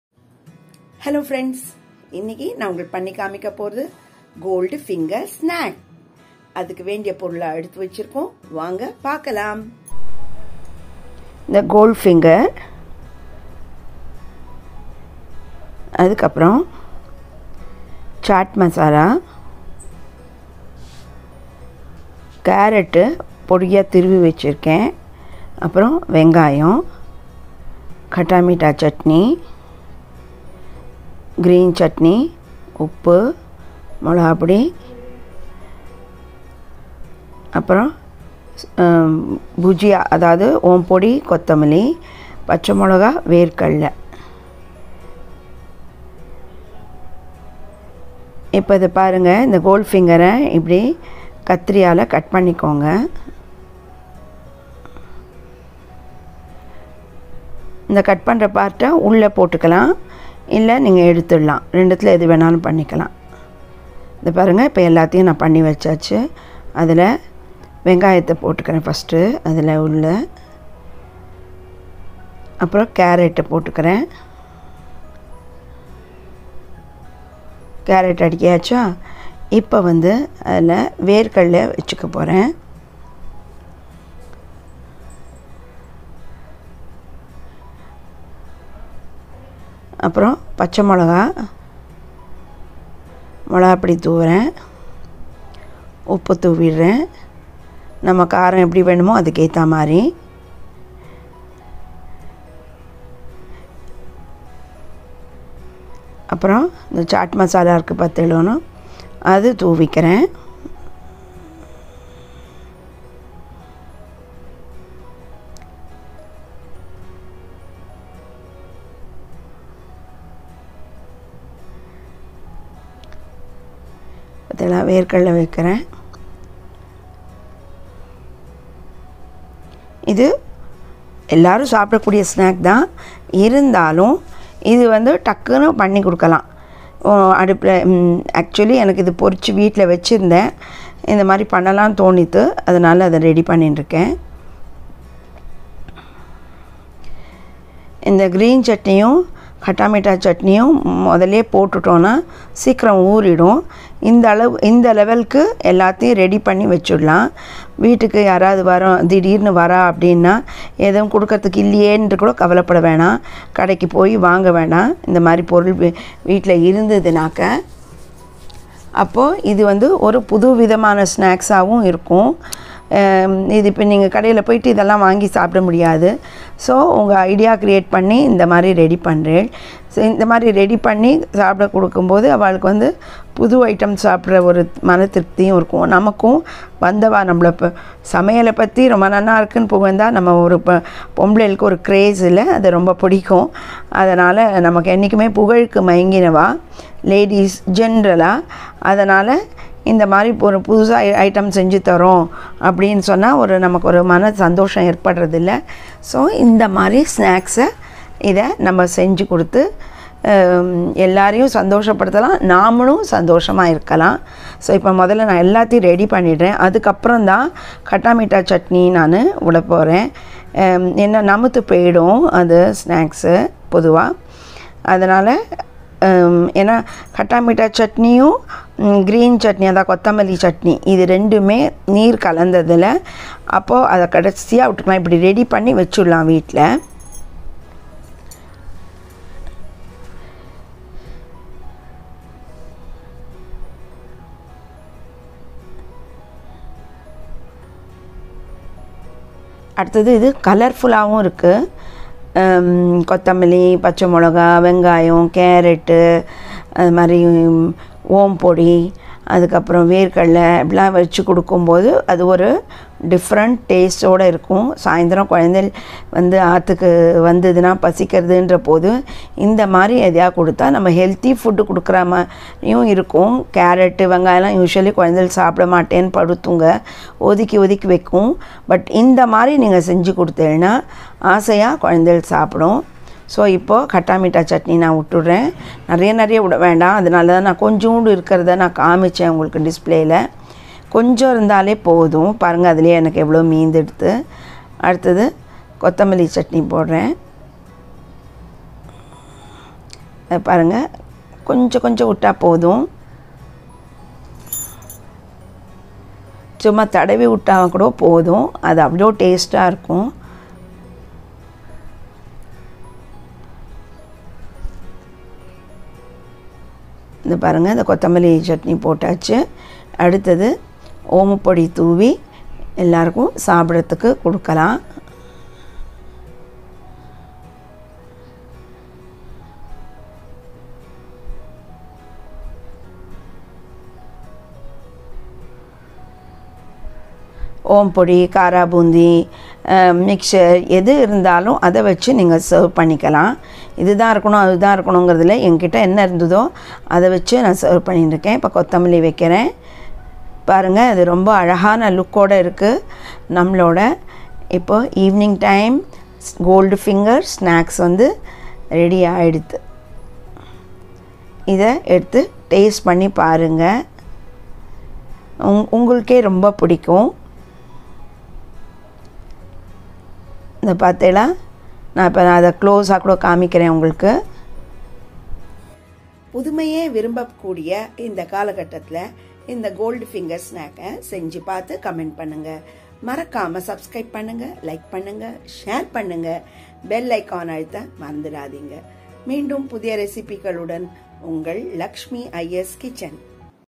Goldfinger snack
1. Goldfinger - 10
2. Carrot - 1/2 cup
3. Onion - 1
4. Chilli powder - 1 tsp
5. Salt - 1 tsp
6. Chat masala - 1 tsp
7. Coriander chatni - 1 tbsp
8. Khatta meetha chatni - 1 tbsp
9. Fried peanut - 1 tbsp
10. Bhujiya sev - 1 tbsp
11. Green chilli - 1
12. Chopped coriander